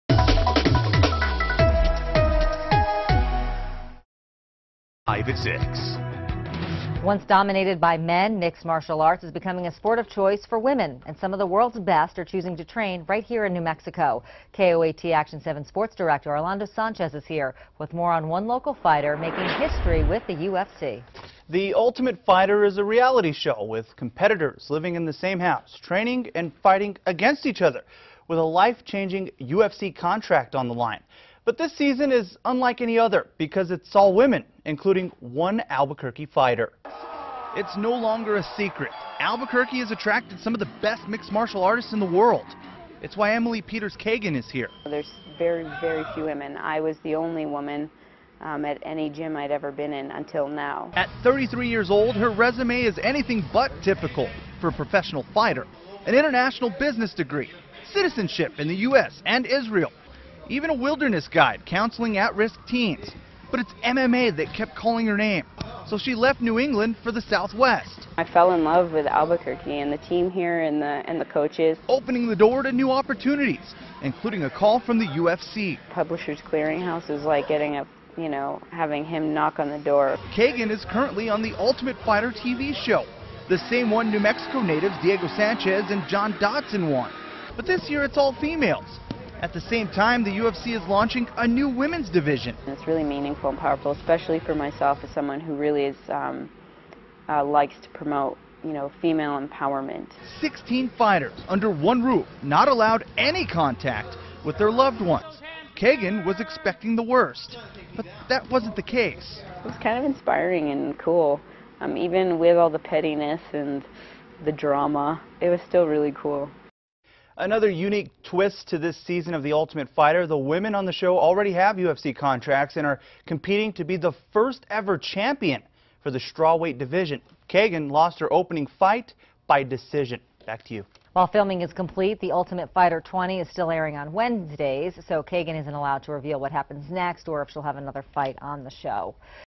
NM fighter part of UFC history
Mixed Martial Arts (MMA) is becoming a sport of choice for women, and some of the world's best are choosing to train here in New Mexico.